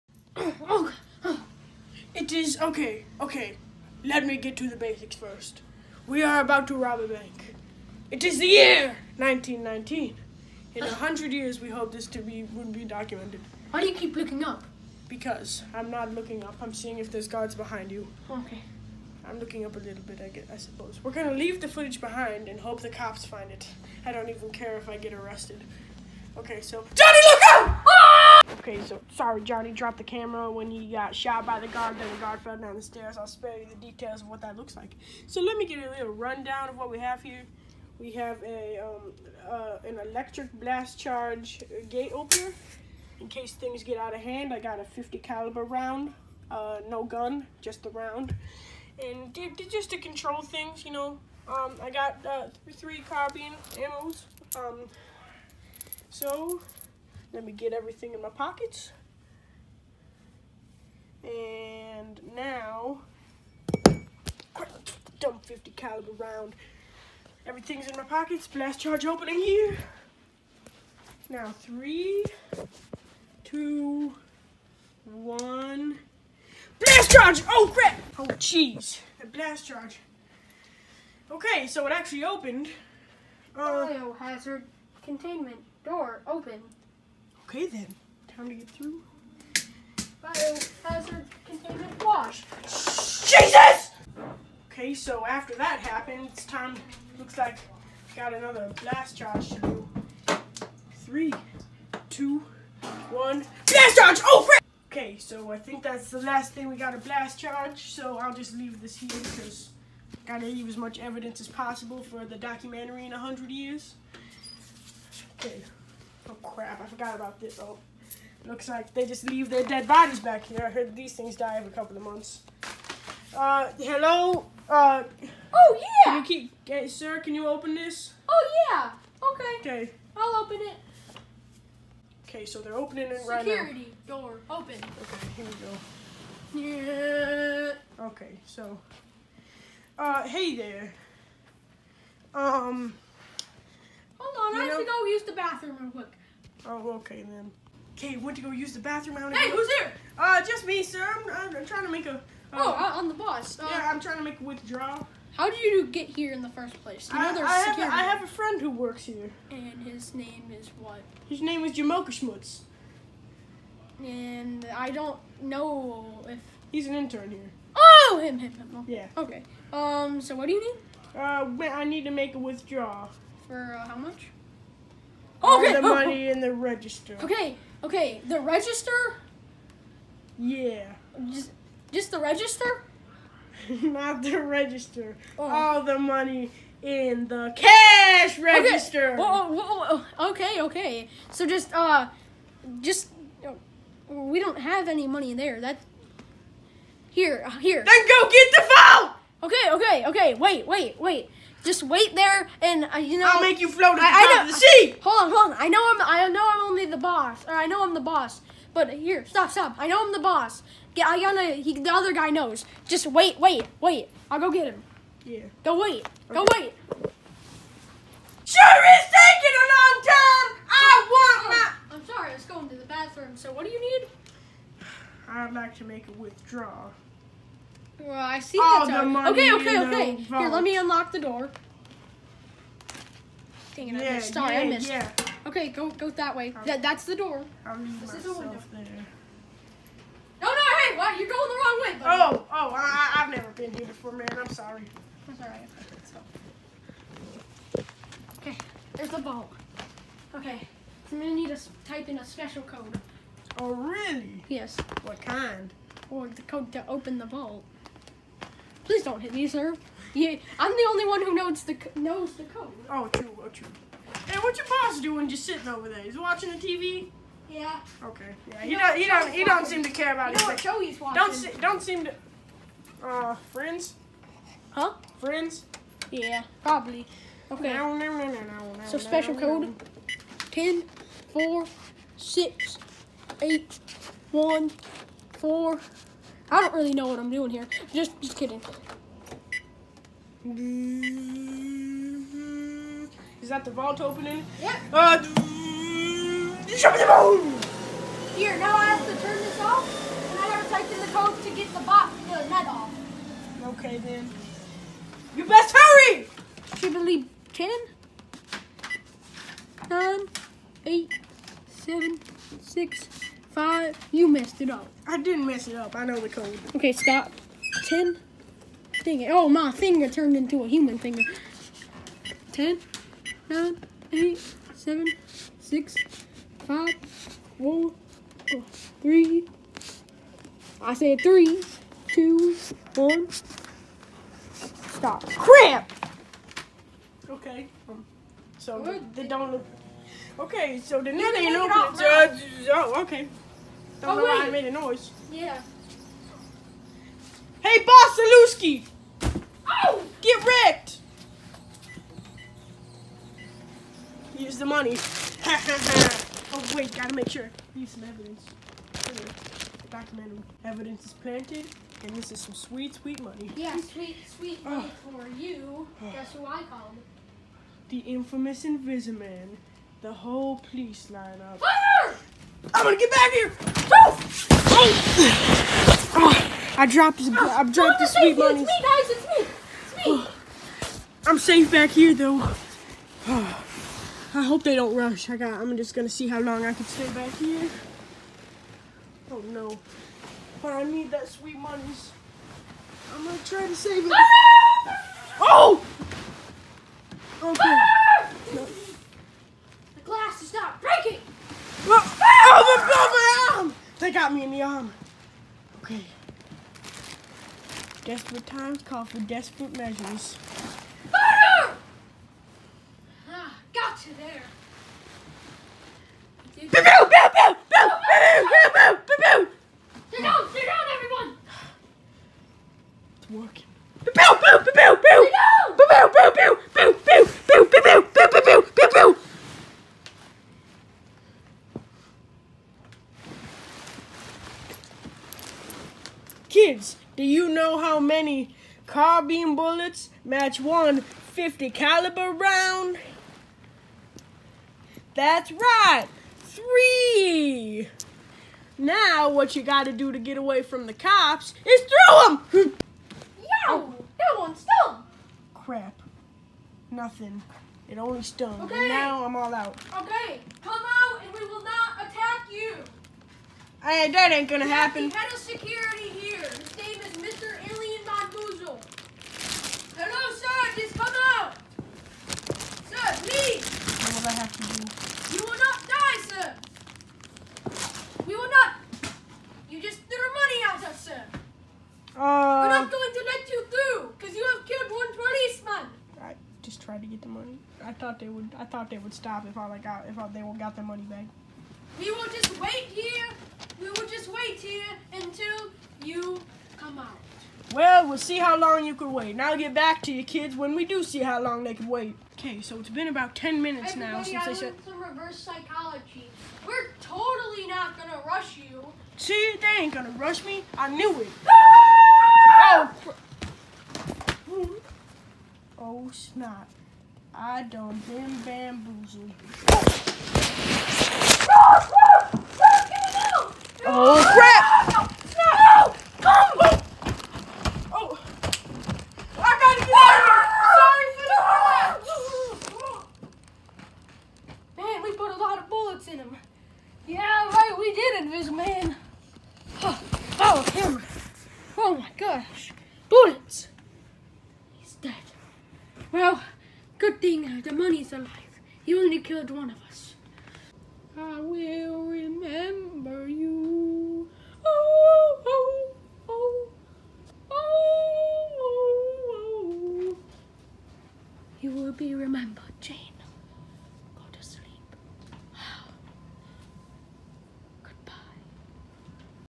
The great bank robbery of 1919 documentary
In 1919 the son of Dick Dickerson, Rick Dickerson and his pal, Johnny Tom, committed a bank robbery at the high security bank AREA 52.  Rick documented the whole robbery. We later found that footage and enhanced it for higher quality. We later released it. Here is that footage...